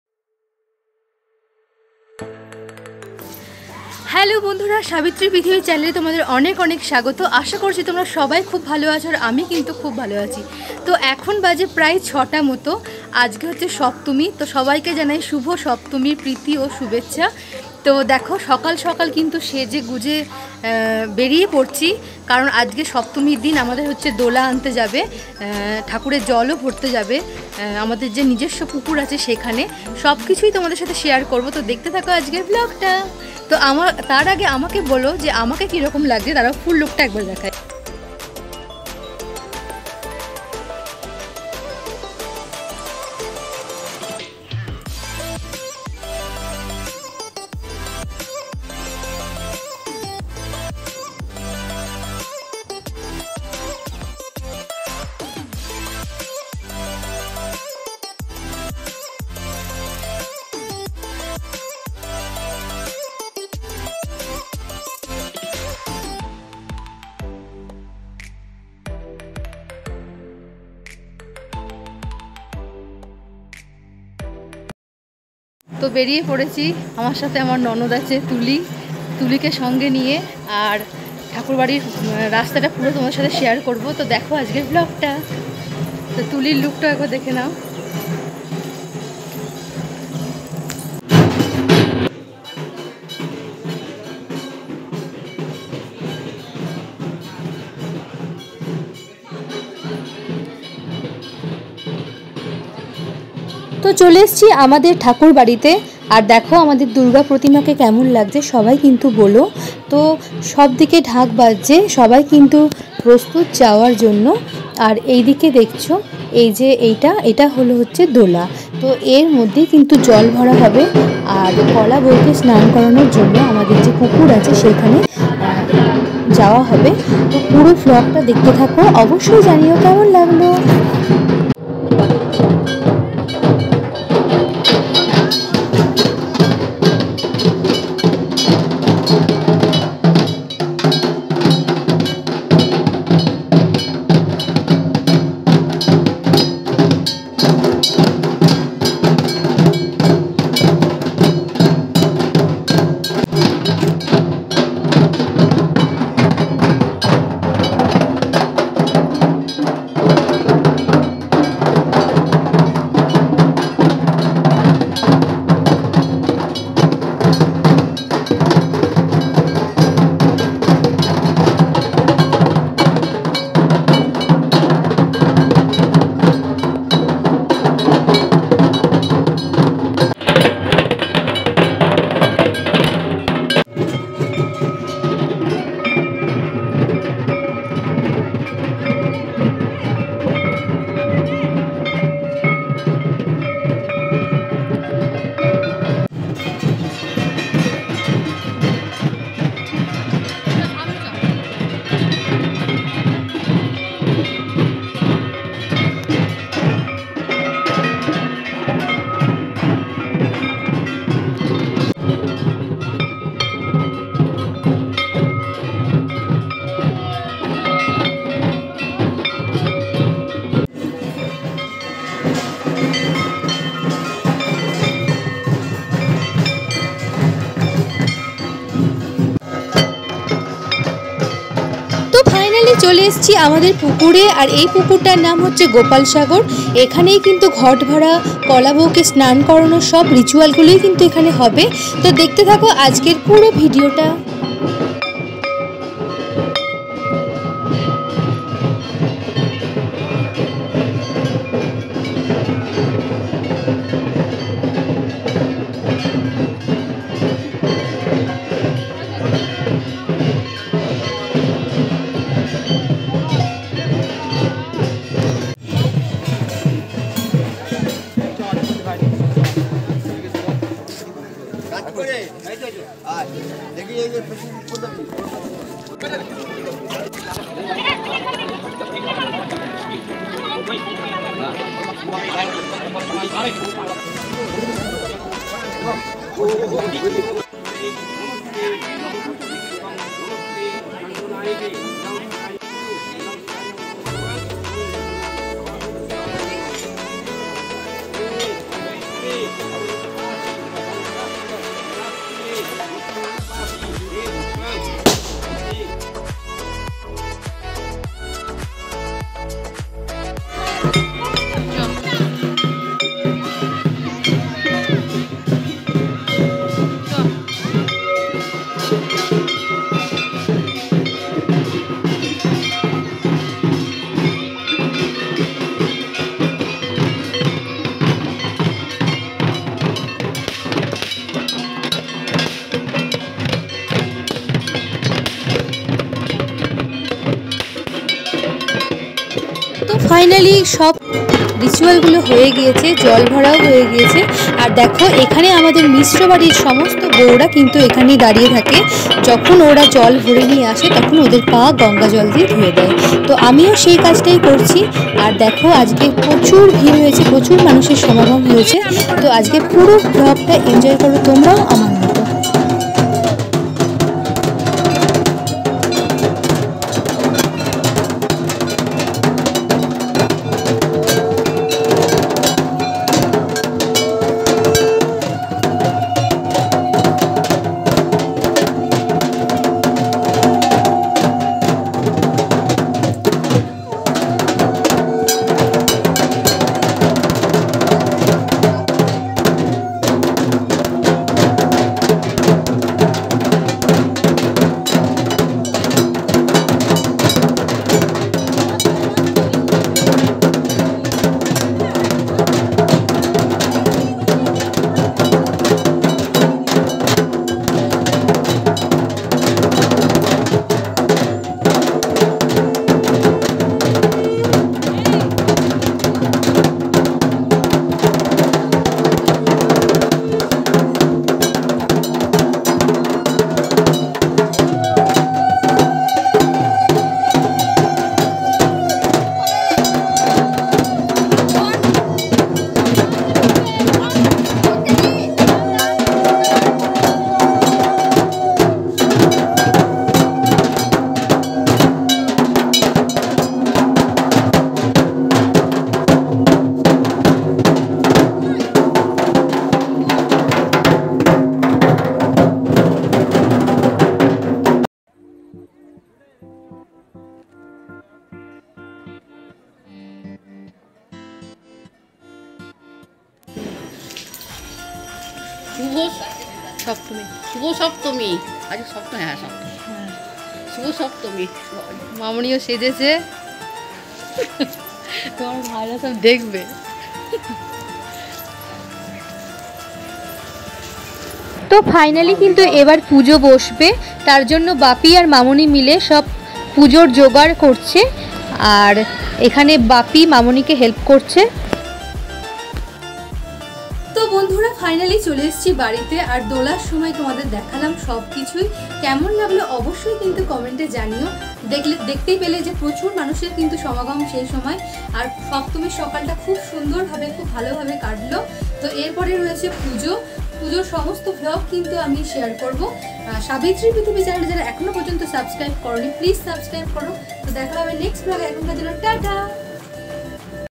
মিশ্র বাড়ির 1500 বছরের প্রাচীন দূর্গাউৎসব/সপ্তমীর সকাল/দোলা যাত্রা/কলা বৌ স্নান/durgapuja 2022
durga puja ritual/জমিদার বাড়ির দুর্গা পুজো